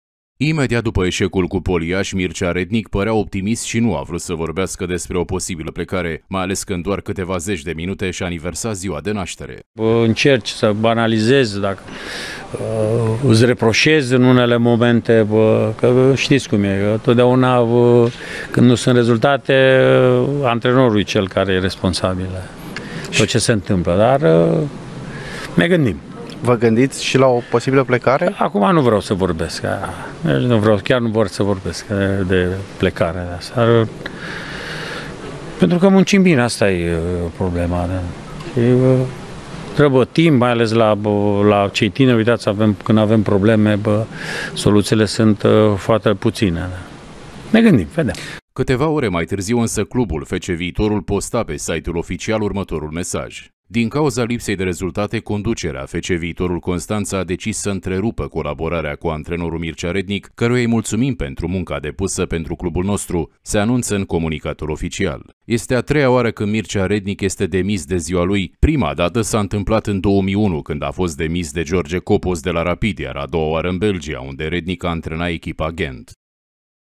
Rednic, dat afară de ziua lui
Imediat după ce a pierdut meciul cu Poli Iași, antrenorul Mircea Rednic a fost dat afară de Viitorul. Rednic a primit vestea cu doar câteva ore înainte de ziua lui de naștere. Fostul antrenor al FC Viitorul împlinește astăzi 59 de ani.